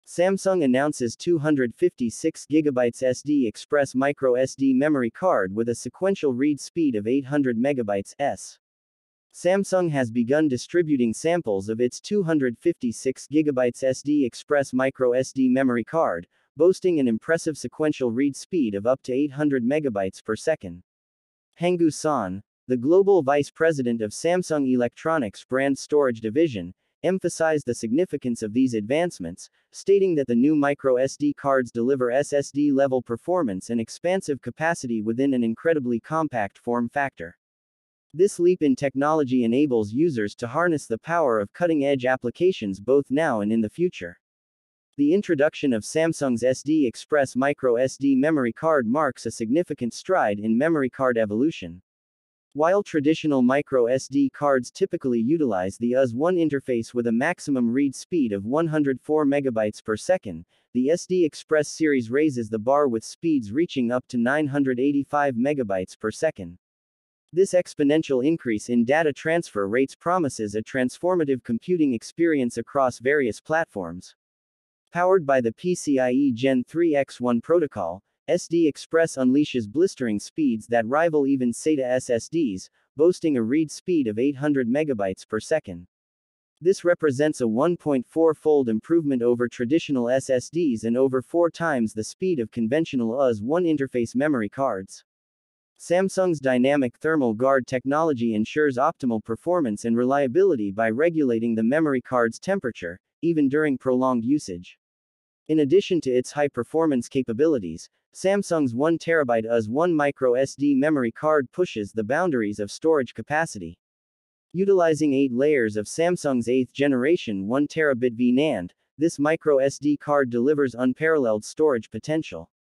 Samsung announces 256GB SD Express microSD memory card with a sequential read speed of 800MB/S.
Samsung has begun distributing samples of its 256GB SD Express microSD memory card, boasting an impressive sequential read speed of up to 800MB/s.